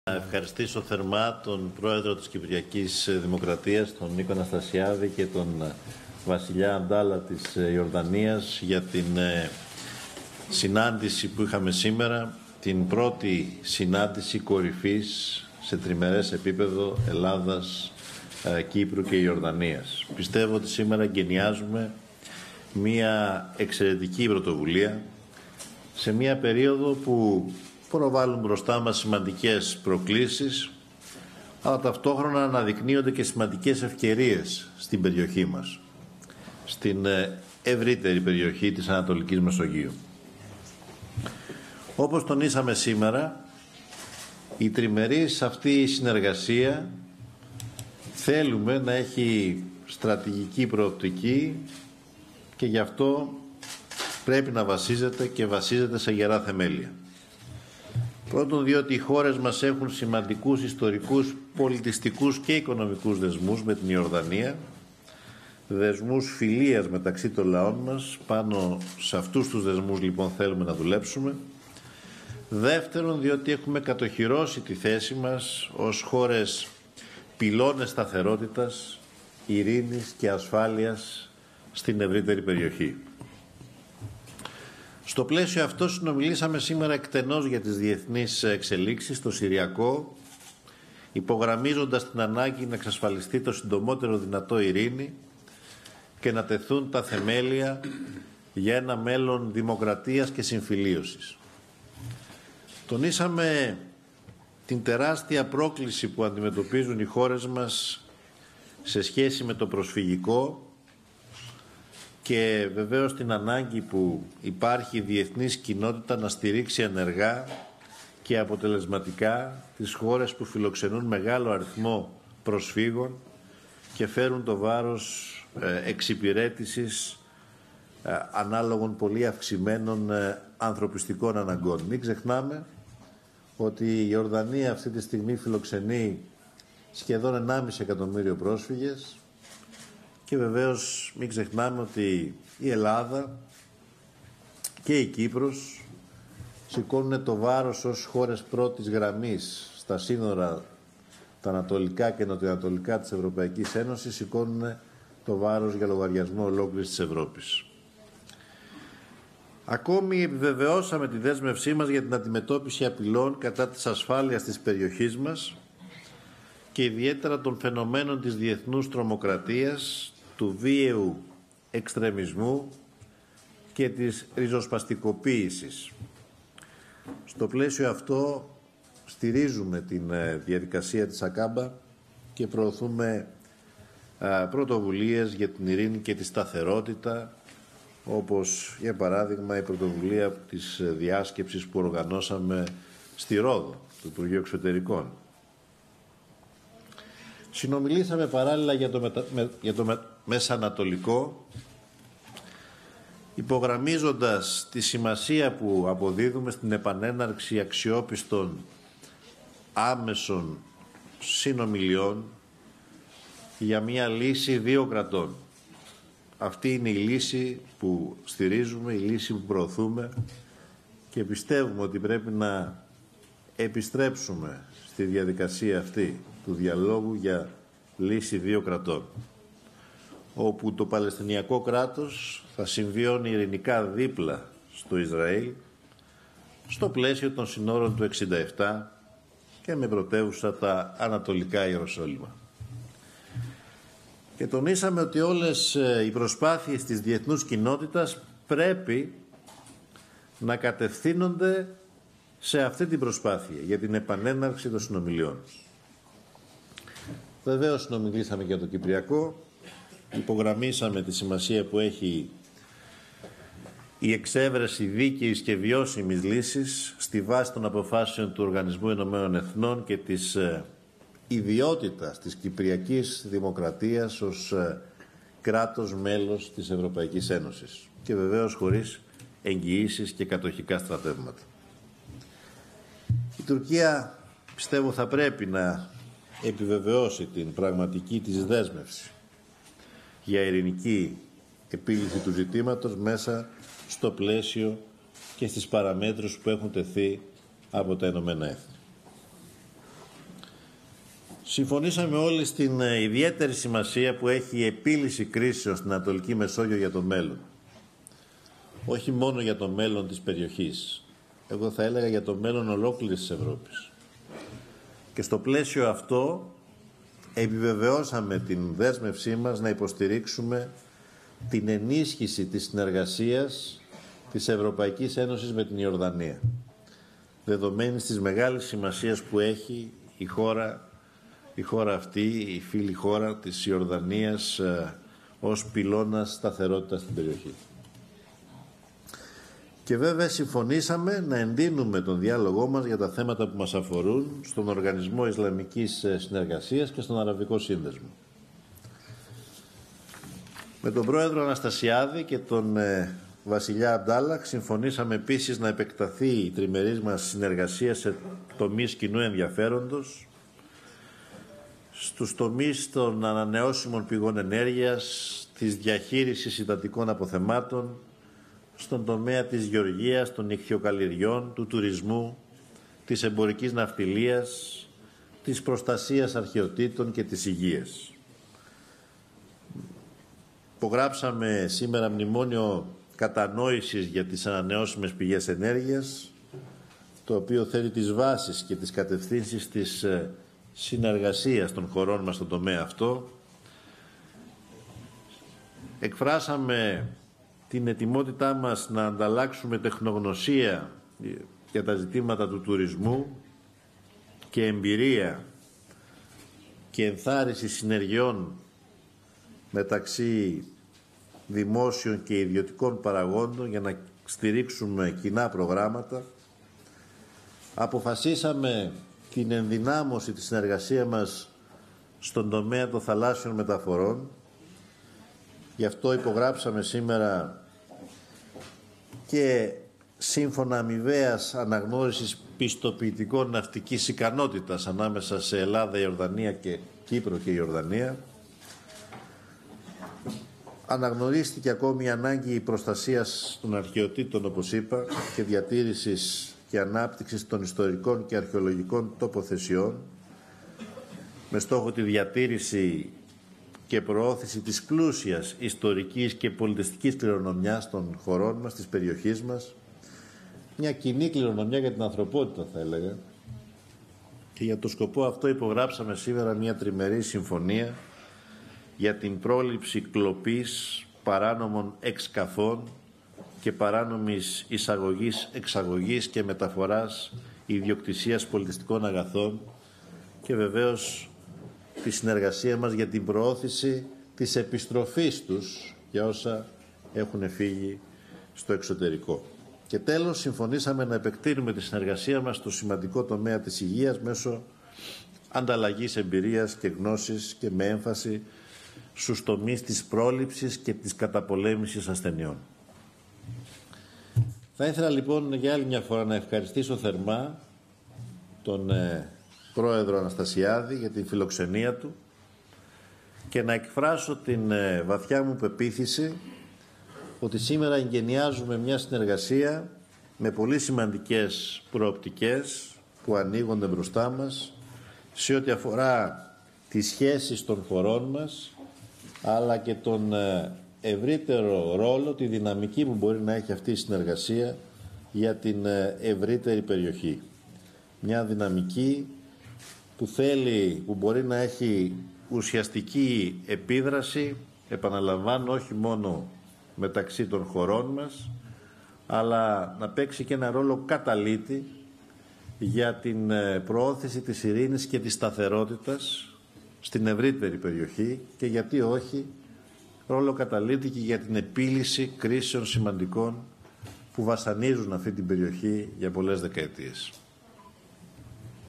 Δηλώσεις στο πλαίσιο της 1ης Συνόδου Ελλάδας - Κύπρου - Ιορδανίας
Κοινές δηλώσεις του  Πρωθυπουργού Αλέξη Τσπίπρα, του Προέδρου της Κυπριακής Δημοκρατίας Νίκο Αναστασιάδη και του Βασιλιά της Ιορδανίας Αμπντάλα Β' στο πλαίσιο της 1ης Συνόδου Ελλάδας - Κύπρου - Ιορδανίας. (Λευκωσία, 16/01/2018)